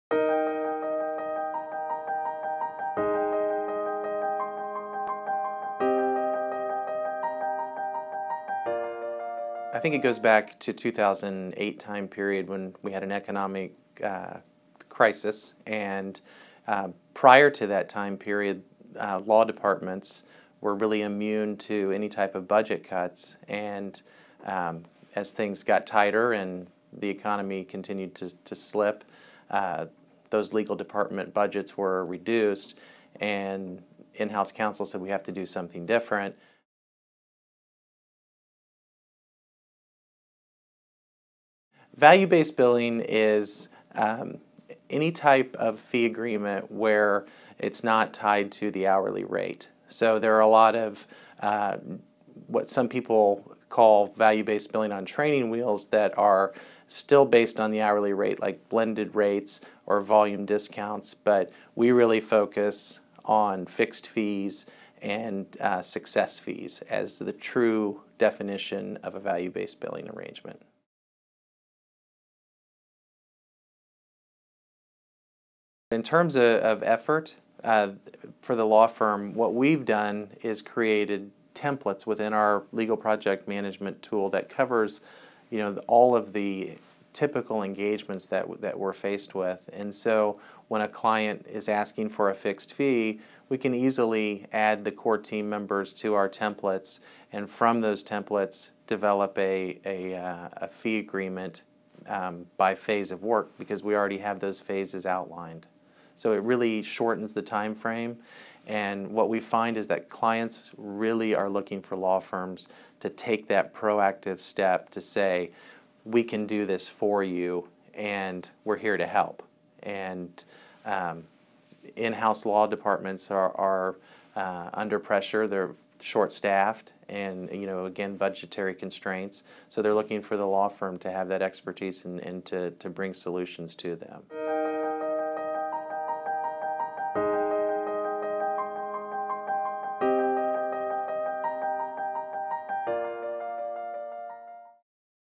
Part 1: Alternative Fees in Law, with Matt Laws of Crowell & Moring LLP
In this three-part video alert series, Crowell & Moring Senior Director of Practice Management, Matt Laws, discusses the evolution of pricing and legal project management, how companies and their law firms can make alternative pricing work, and the growth and future of alternative fees in legal work. Matt covers several best practices to advance the use of AFA’s for in-house legal departments, focusing primarily on fixed fees and success-based pricing options.